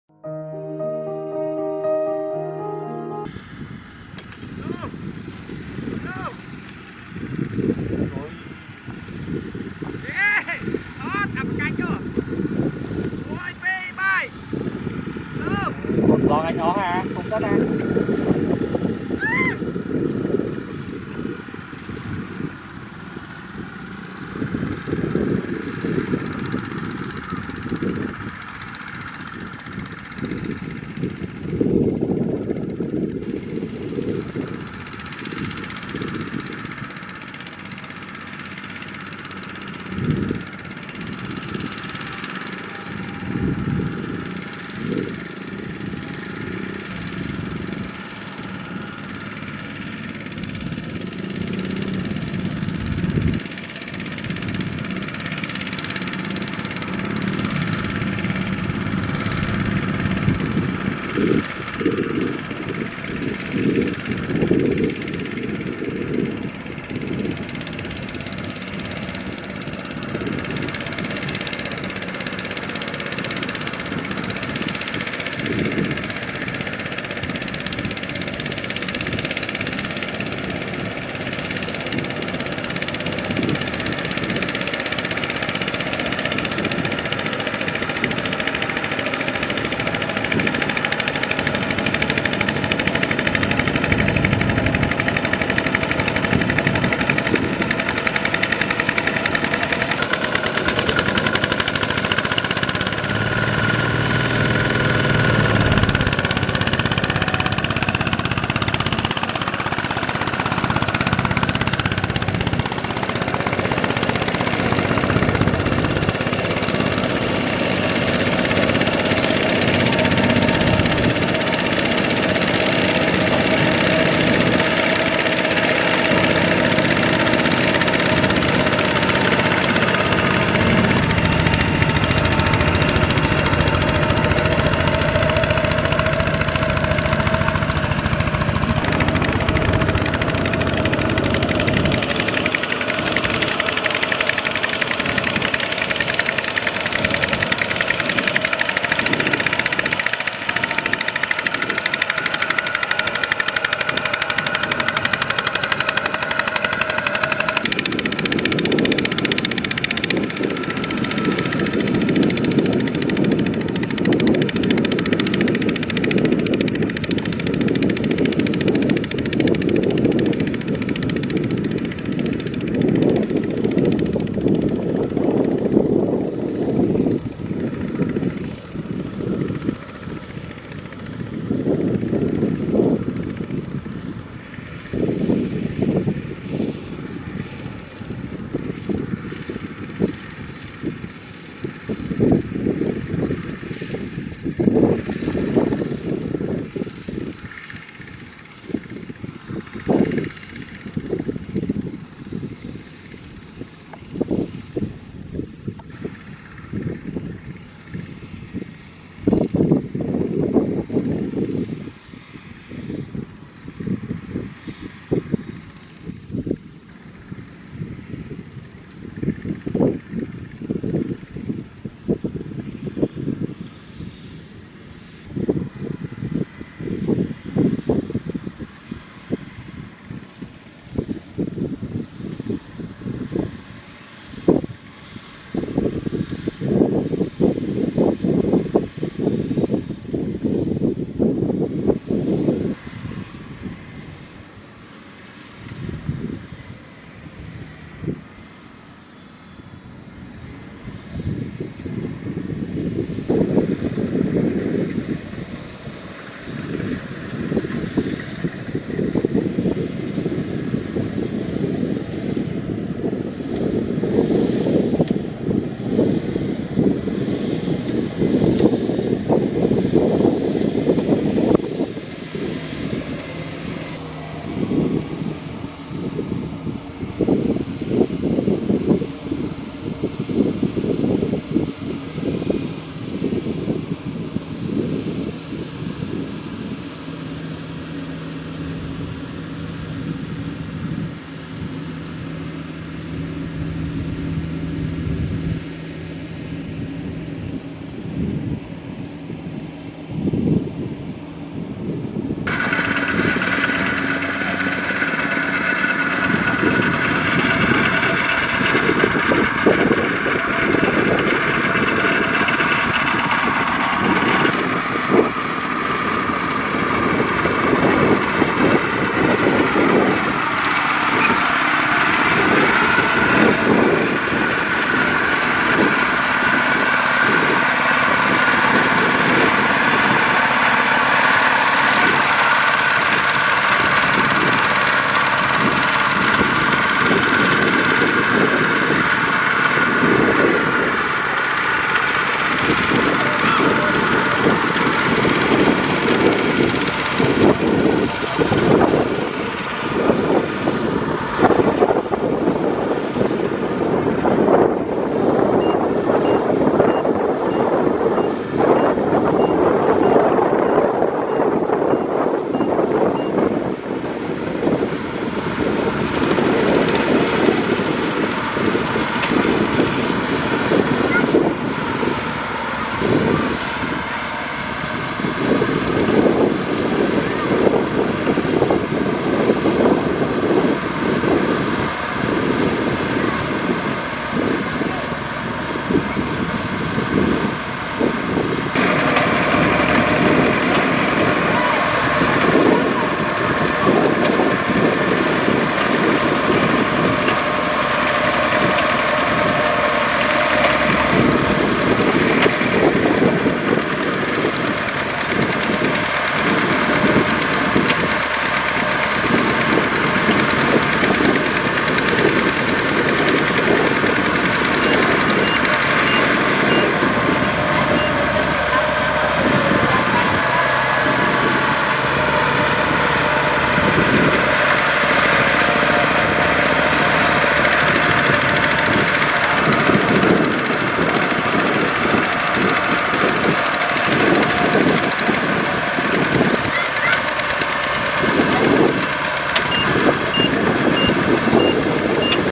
Powerful of tractor RT120 in transporting rice performance | គោយន្តដឹកស្រូវកាត់ផ្លូវលំបាក
សូមរីករាយទស្សនាសកម្មភាពគោយន្តដឹកស្រូវកាត់ផ្លូវលំបាក
-Tractor Kubota RT120
-Tractor Kubota ZT155

Thank you very much for supporting and watching my videos. Hope you enjoy and have a great time watching our videos. Thanks a lot.